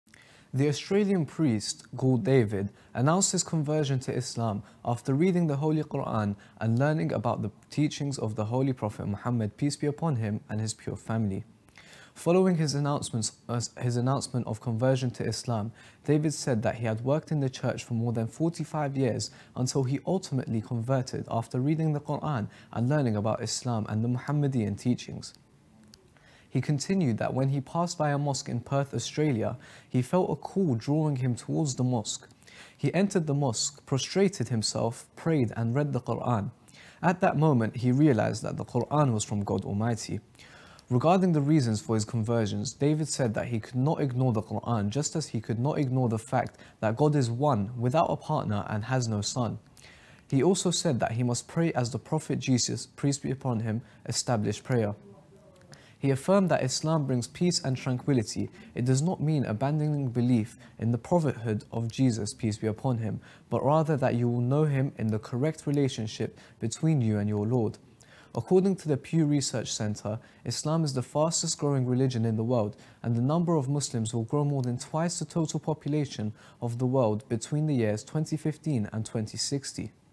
Australian priest, Gould David, announced his conversion to Islam
Australian priest Gould David converts to Islam, citing Quranic teachings and Prophet Muhammad's example. He felt drawn to a mosque, prayed, and embraced Islam, emphasizing the oneness of God. He finds peace and emphasizes the growth of Islam globally.

The Australian priest "Gould David" announced his conversion to Islam after reading the Holy Quran and learning about the teachings of the Holy Prophet Muhammad, peace be upon him and his pure family.
Following his announcement of conversion to Islam, David said that he had worked in the church for more than 45 years until he ultimately converted after reading the Quran and learning about Islam and the Muhammadian teachings.
He continued that when he passed by a mosque in Perth, Australia, he felt a call drawing him towards the mosque. He entered the mosque, prostrated himself, prayed, and read the Quran. At that moment, he realized that the Quran was from God Almighty.
Regarding the reasons for his conversion, David said that he could not ignore the Quran, just as he could not ignore the fact that God is One, without a partner, and has no son. He also said that he must pray as the Prophet Jesus (peace be upon him) established prayer.

He affirmed that Islam brings peace and tranquility. It does not mean abandoning belief in the prophethood of Jesus, peace be upon him, but rather that you will know him in the correct relationship between you and your Lord.
According to the Pew Research Center, Islam is the fastest-growing religion in the world, and the number of Muslims will grow more than twice the total population of the world between the years 2015 and 2060.